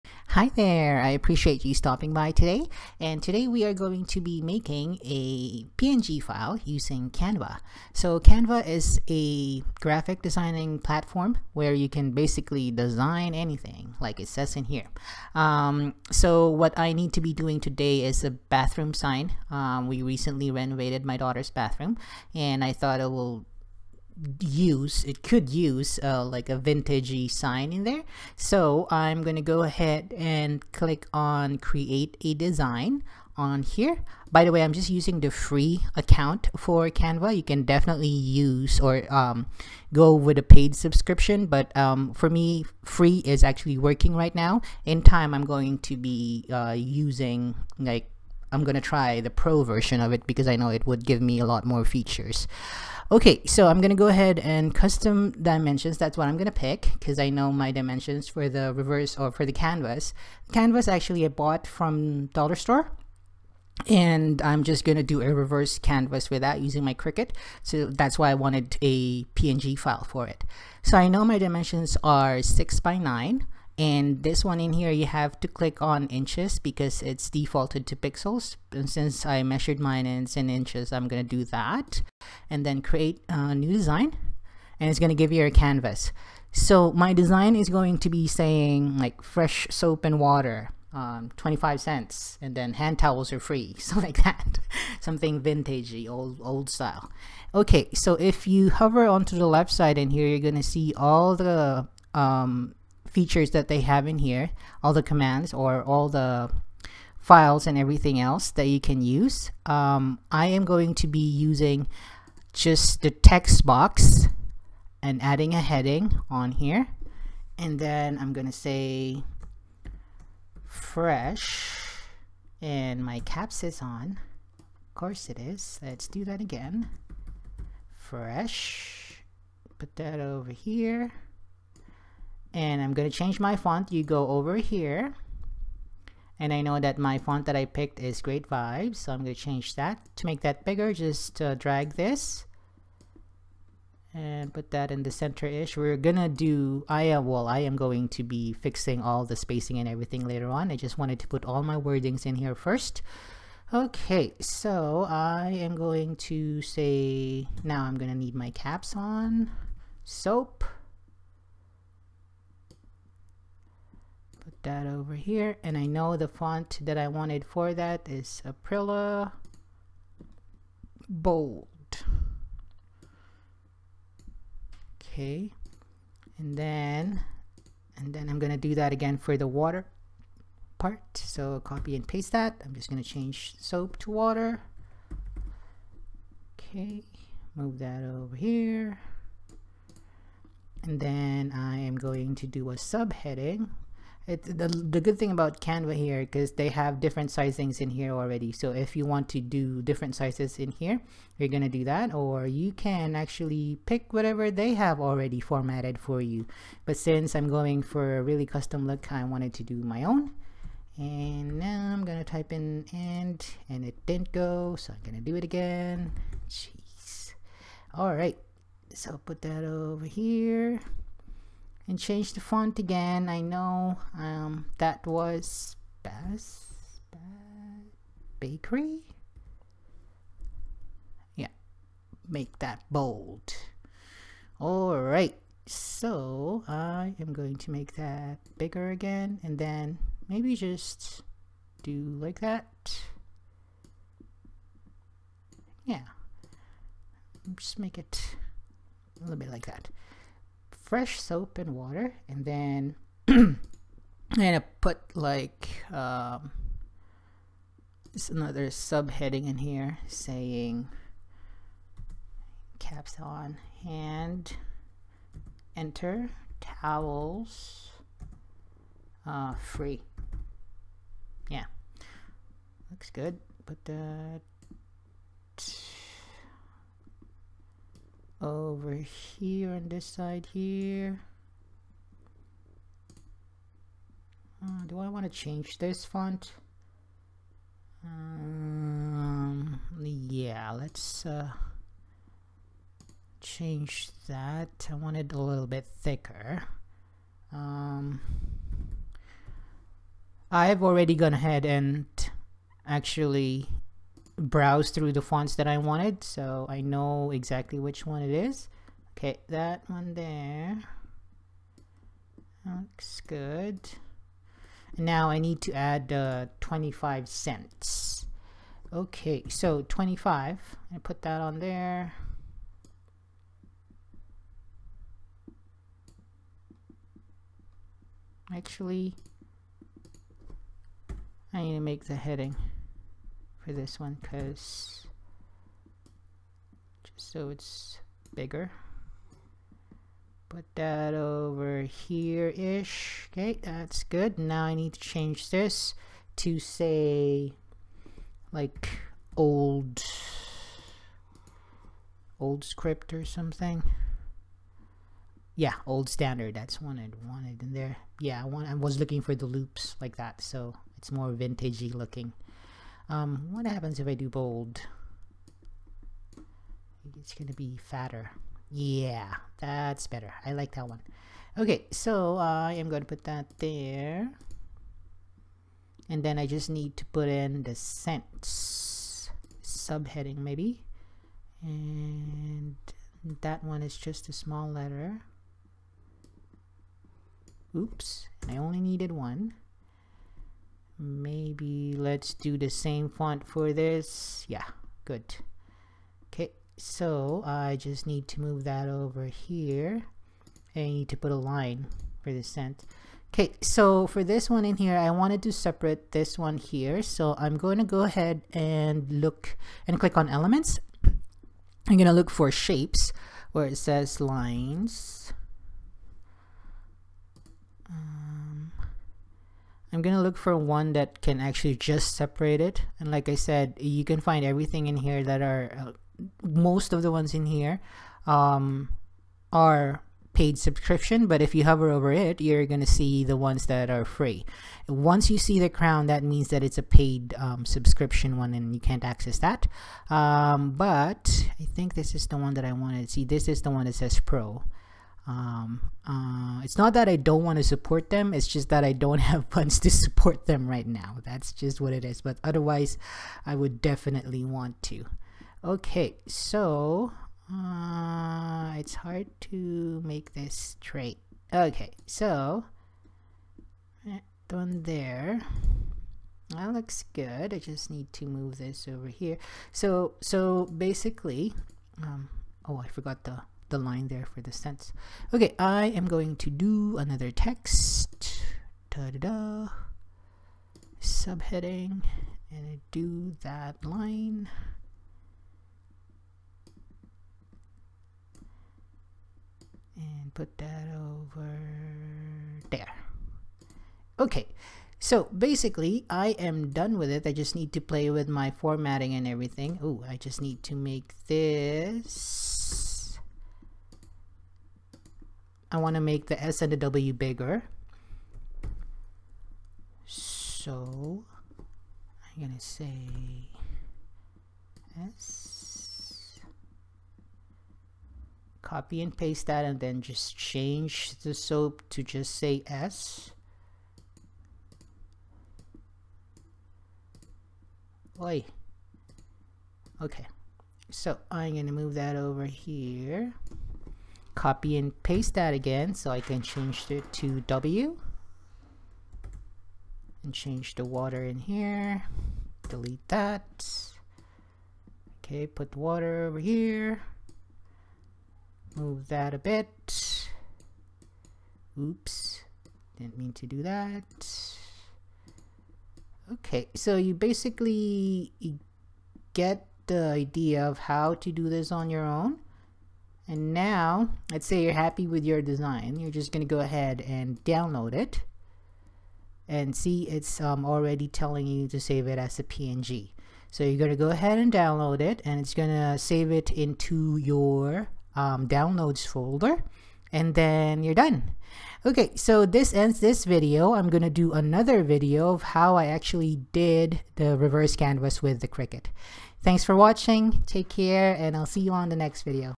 HOW TO CREATE YOUR OWN PNG FILE USING CANVA | CRICUT DIY BATHROOM SIGN
Thank you for stopping by!  In today's video, we are gong to be making a free PNG file that you design yourself using Canva.com!  It is an awesome graphic designing platform where you can design anything that you want or even use pre formatted ones they have created for you.

I am going to be making another video of how to finish this project using my Cricut so watch out for that!